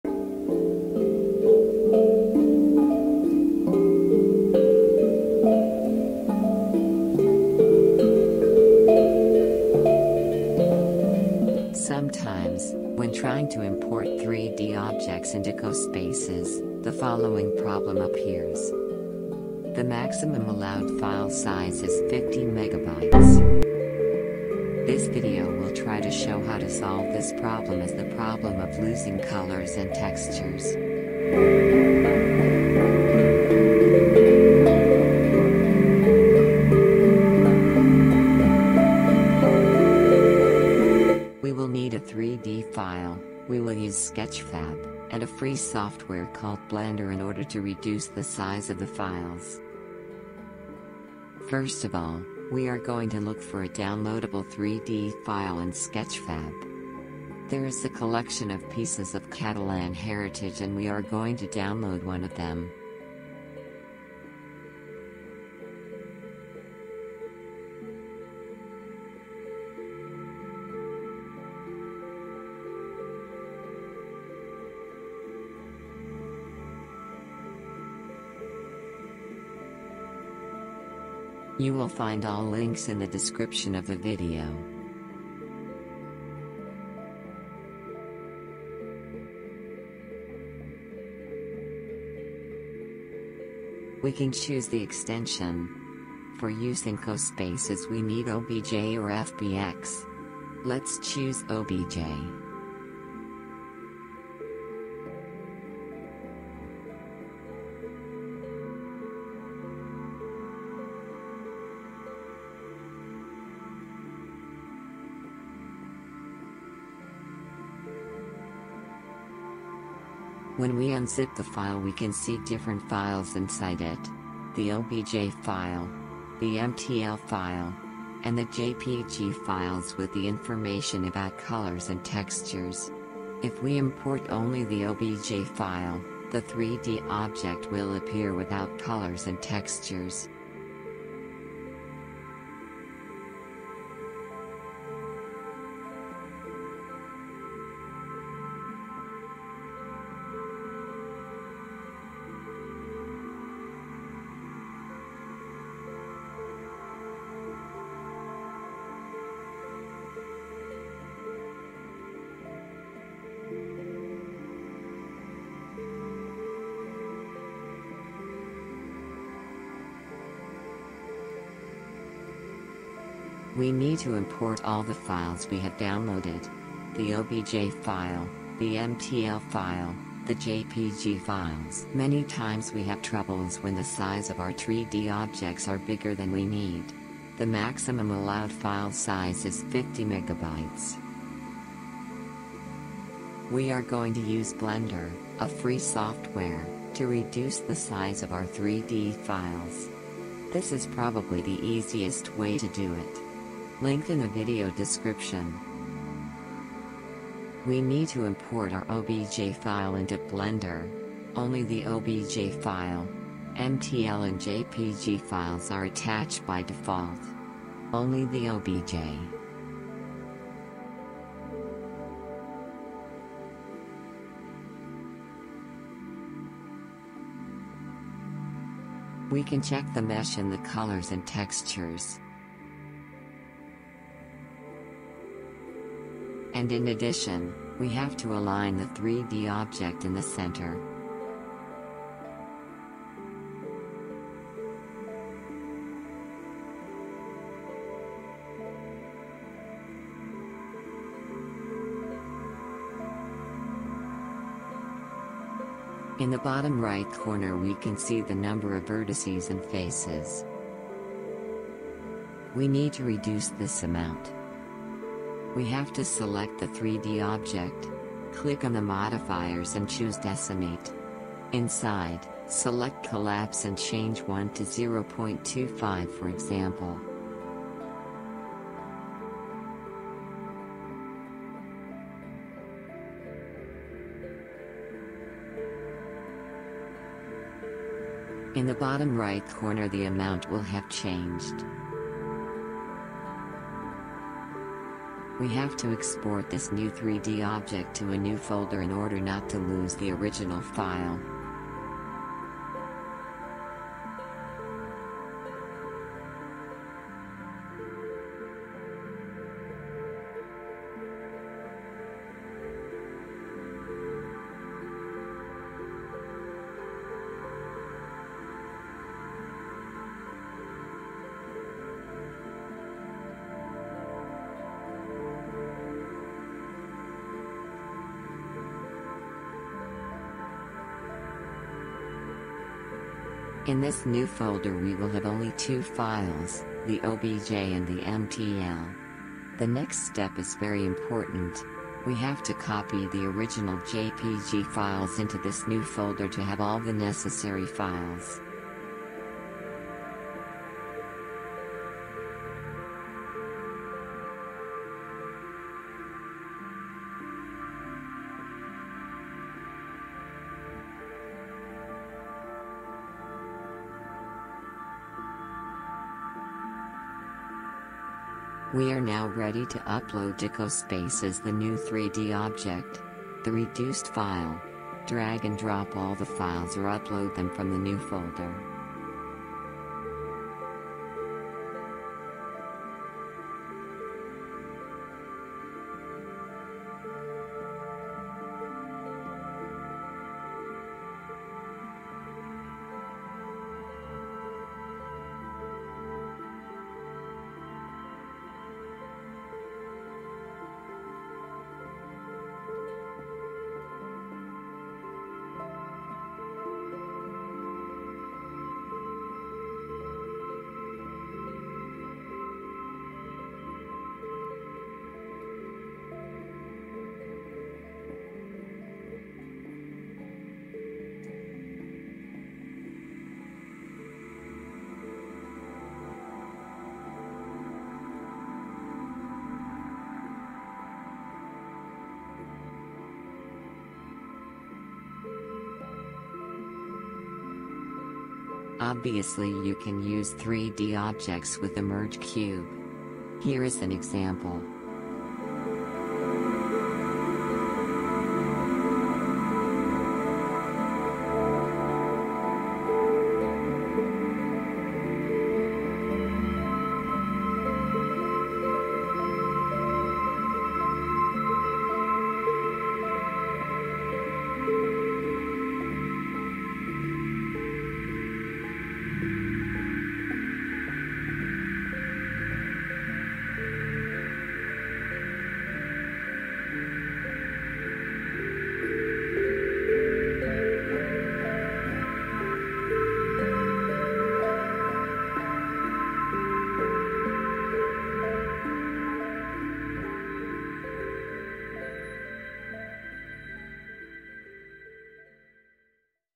Upload 3D objects in Cospaces Size File, Colours and Textures
In this video, we will show how you can change the size of 3D objects, how to reduce size with Blender, and how to preserve colors and textures.

Jesús Arbués @JesusArbues